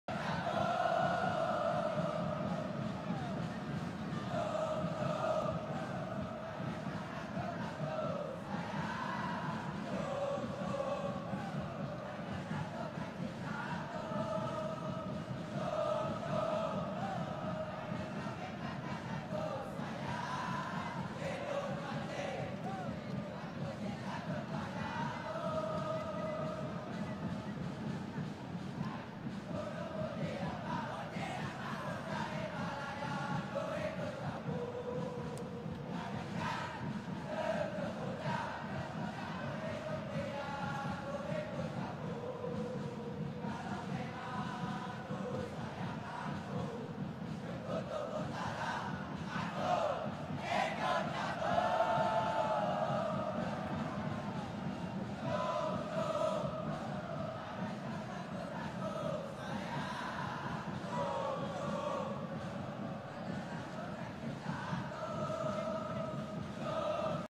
Highlight Majlis Perasmian Stadium Sultan Ibrahim | Stadium baharu Johor Darul Ta'zim (JDT)
EnjoyGalaxy has the great honor to light up the audiences, bring them to be part of the history moments.

30,000 Enjoygalaxy led balls were used.